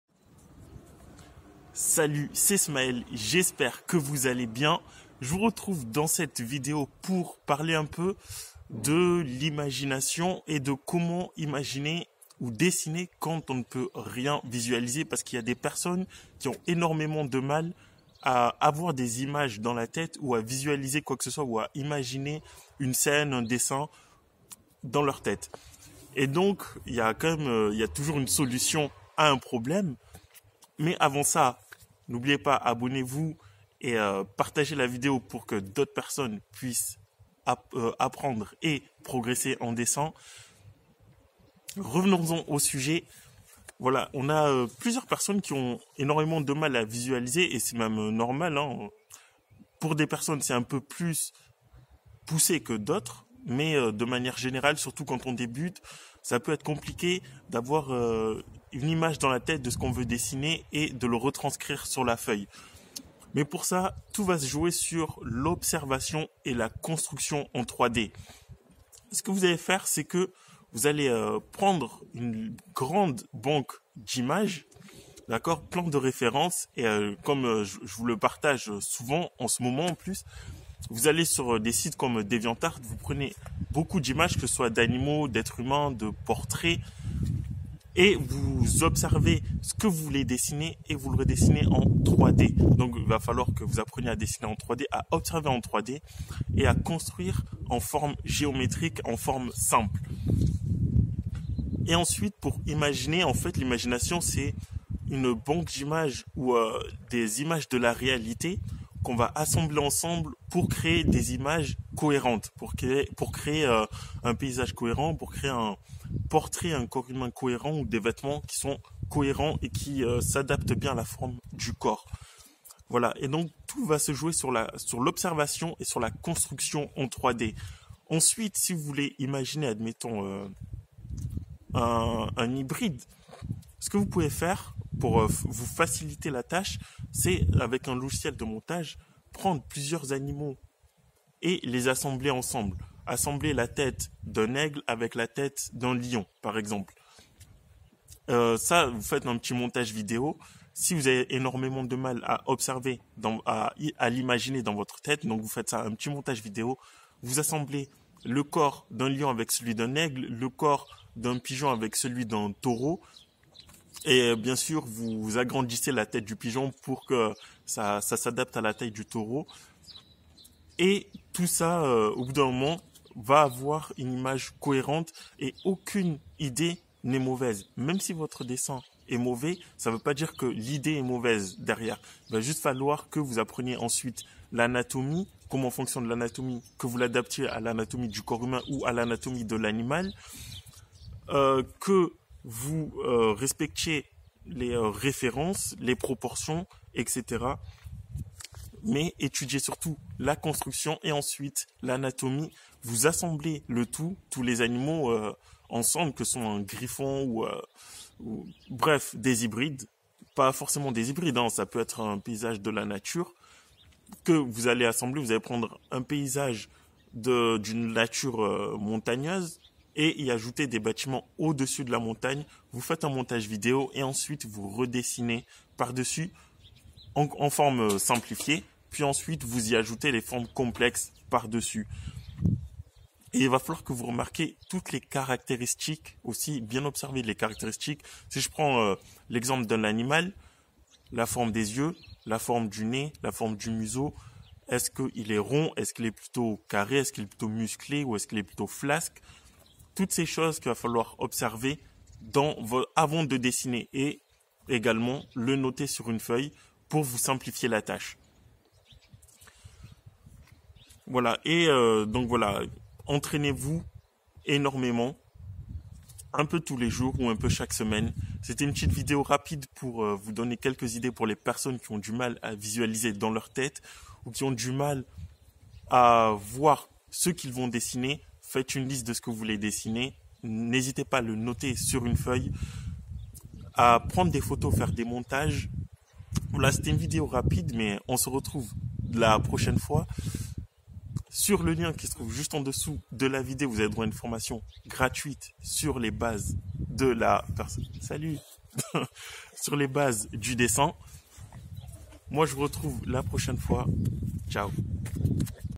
Dessiner quand on ne peut rien visualiser !
dessiner quand on ne peut rien visualiser Dans cette vidéo je vais vous apprendre comment dessiner quand vous avez du mal à visualiser des choses.